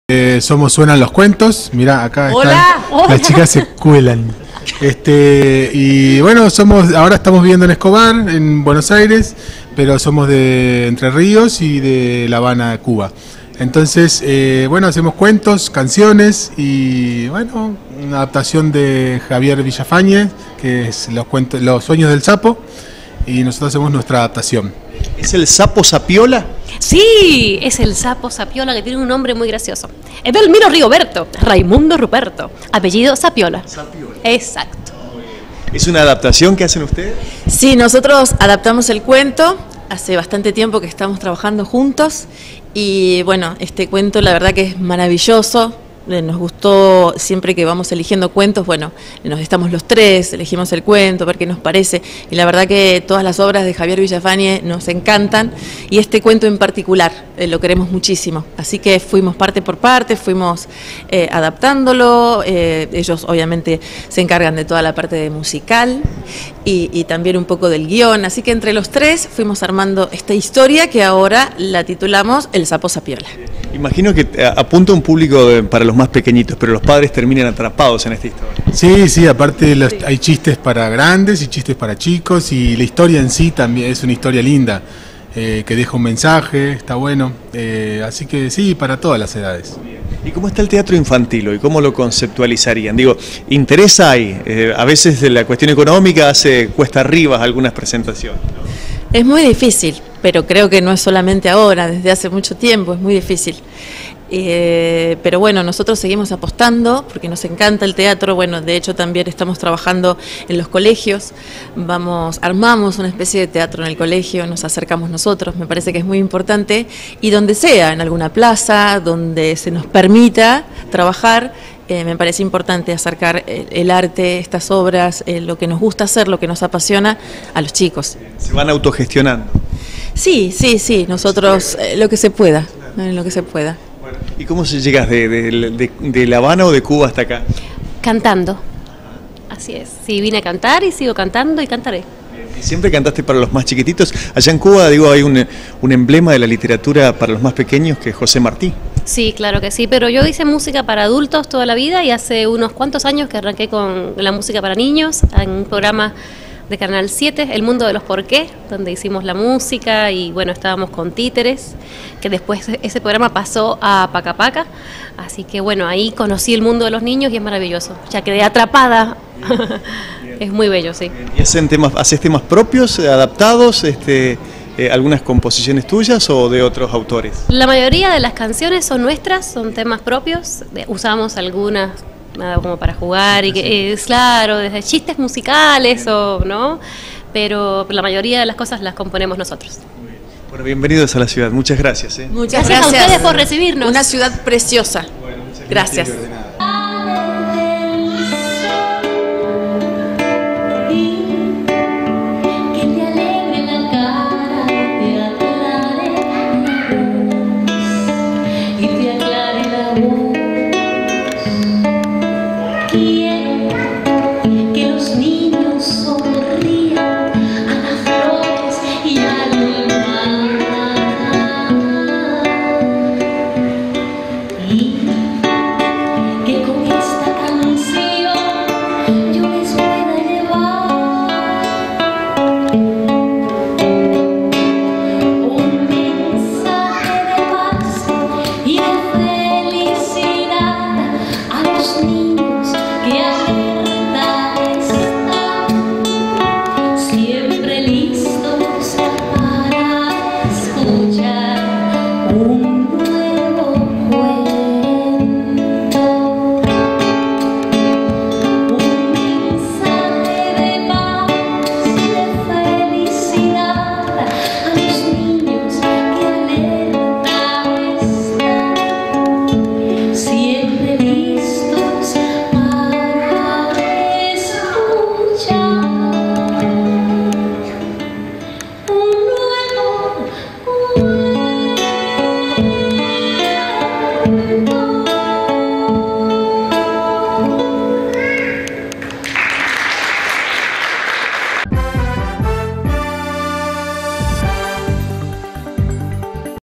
Obra teatral infantil - El Sapo Sapiola
Cerraron las vacaciones de julio en el Cine Rivadavia